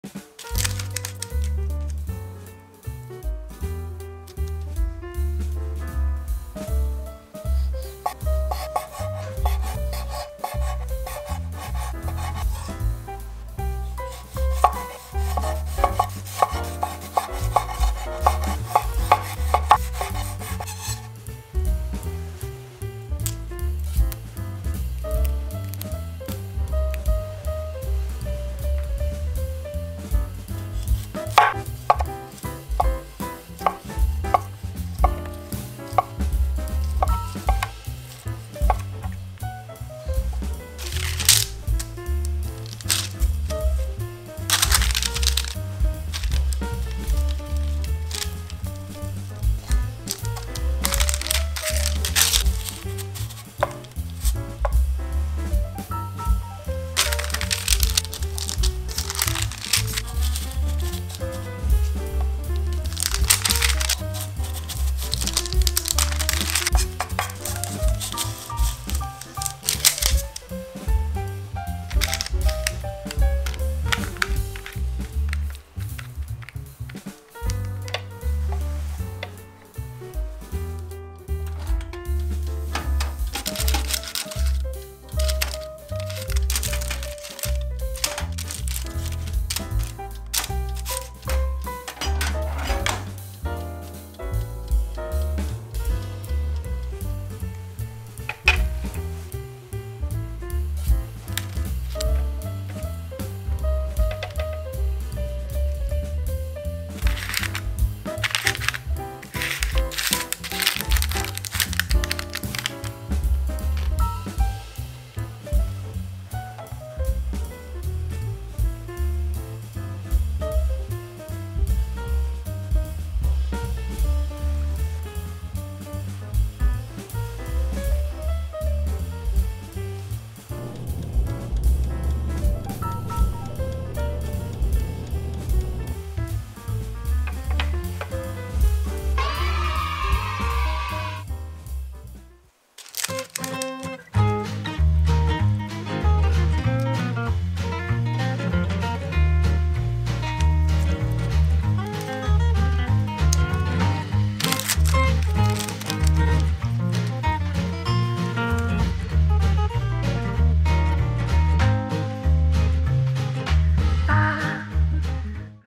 노 밀가루!! 고구마1개로 피자한판 만들어 드세요😁 바삭하고 촉촉한! 에어프라이어 레시피!
자막이 자동으로 생성되면 CC버튼을 눌러 자막을 꺼주세요😊

오늘의 준비물!

- 고구마 1개
- 방울토마토
- 올리브
- 파슬리
- 모짜렐라치즈
- 토마토 스파게티 소스
- 올리브오일

❤️종이호일 안에 고구마를 넣고 누르면 안들러붙어요
💙고구마는 180도 10분 (노릇노릇하게) 굽고 뒤집어서 5분 구워줍니다
💛토핑을 올리고 180도 5분 (치즈만 녹여주세요)
🧡원하는 토핑을 올려주세요
💚맛있게드세요

Today's Ingredients

- 1 sweet potato
- Cherry tomato
- olive
- Parsley
- mozzarella cheese
- Tomato Spaghetti Sauce
- Olive oil